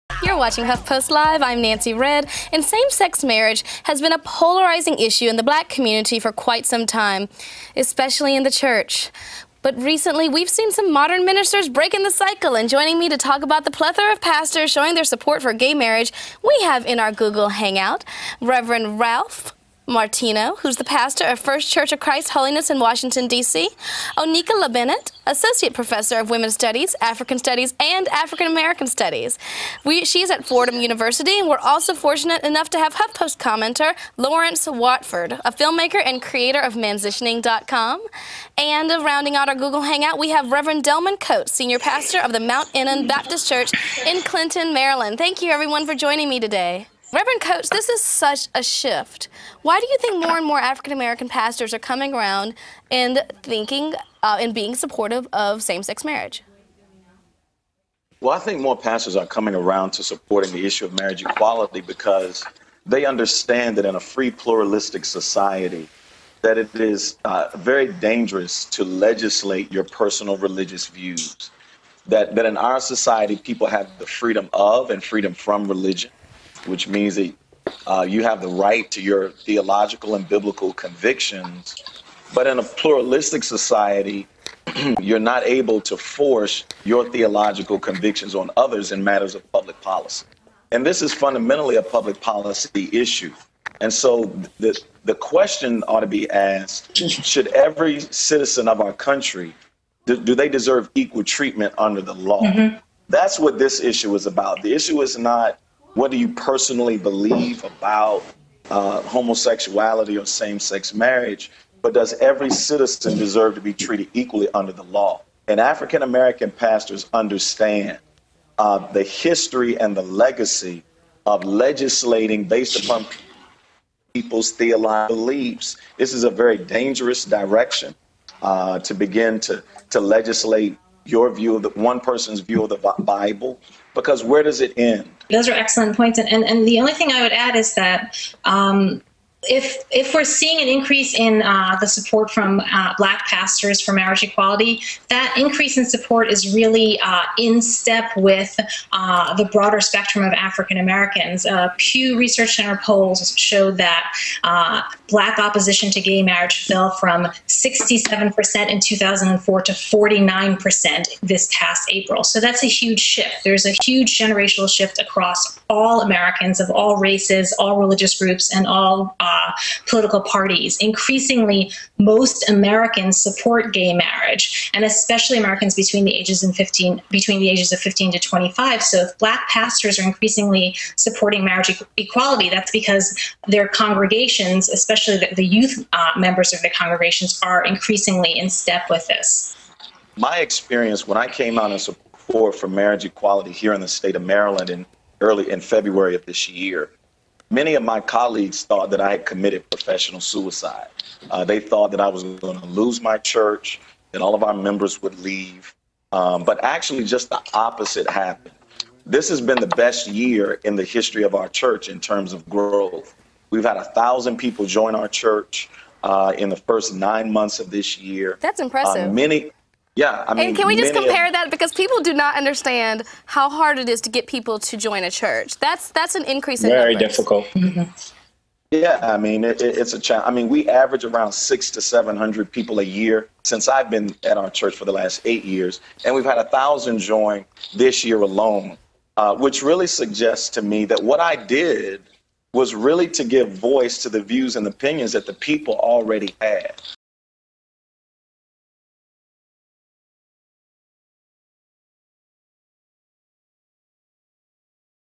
Shifting Same-Sex Views - The Unlikely Endorsement
Same-sex marriage, once a divided issue in the African-American community, is now seeing some unlikely endorsements: pastors! Nancy Redd is joined by guests Rev. Ralph Martino, Oneka LaBennett, Lawrence Watford, Rev. Amos Brown and Rev. Delman Coates to discuss.